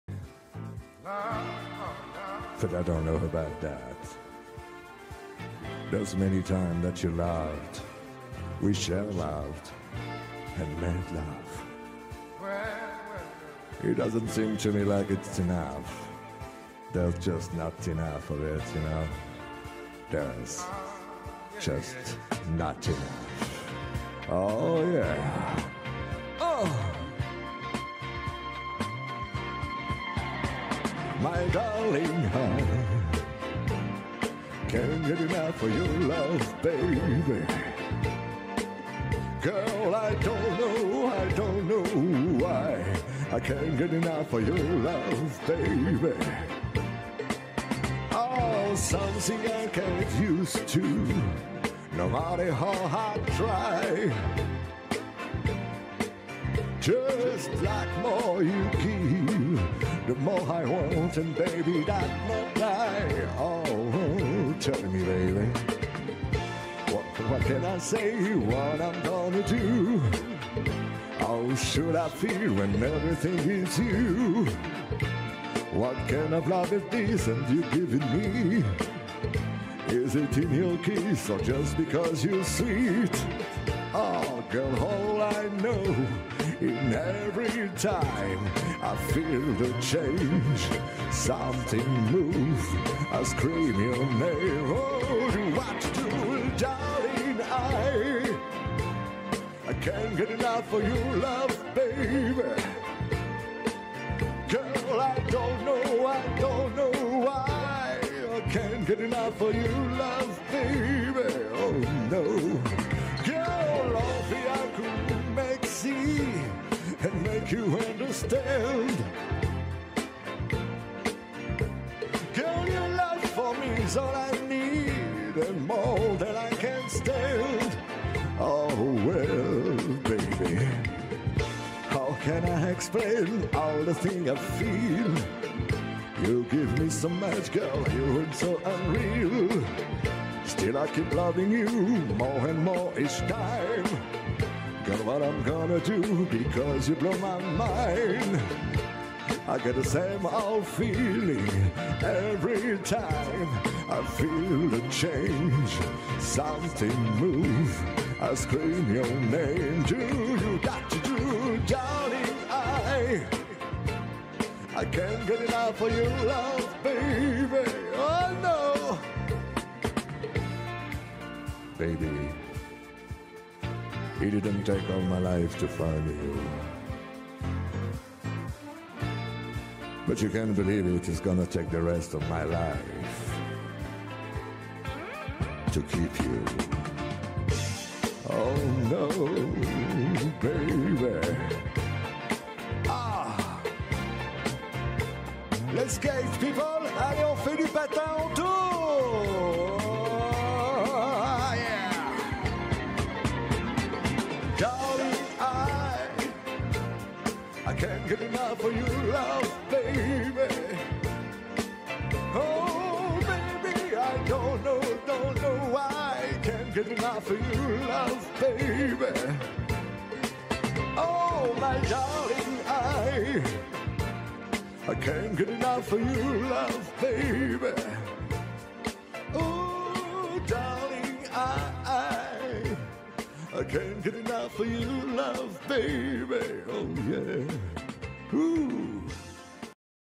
SL-Danse sur une jolie sim
Danse sur la superbe voix  de Byron kiss